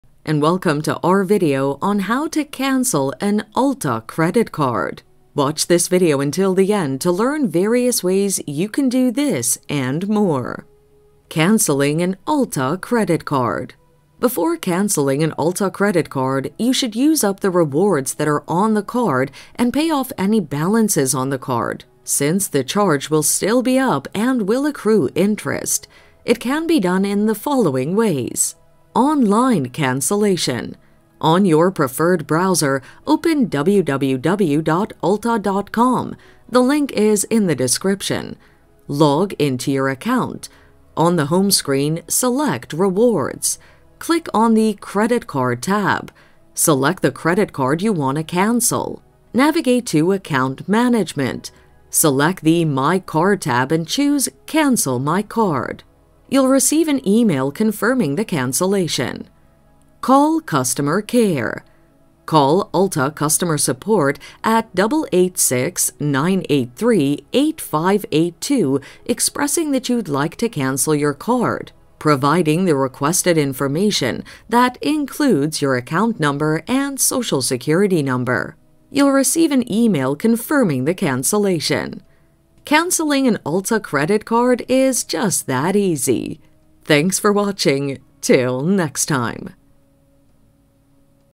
How To Cancel Ulta Credit Card (How Do I Cancel My Ulta Credit Card)
How To Cancel Ulta Credit Card (How Do I Cancel My Ulta Credit Card). In this video tutorial I will show you how to cancel Ulta credit card.

Helpful Links and Content:

Cancelling an Ulta Credit card
Before canceling an Ulta credit card you should use up the rewards that are on the card and pay off any balances on the card since the charge will still be up and it will accrue interest. It can be done in the following ways;

Online cancellation
1. On your preferred browser, open {The link is in the description}.
2. Log in to your account.
3. On the home screen, select rewards.
4. Click on the credit card tab.
5. Select the credit card you want to cancel.
6. Navigate to account management.
7. Select the My Card tab and choose Cancel my card.
8. You will receive an email confirming the cancellation.

Call customer care.
Call Ulta customer support at {The phone number is in the description} expressing that you would like to cancel your card, providing the requested information that includes your account number and social security number. You will receive an email confirming the cancellation.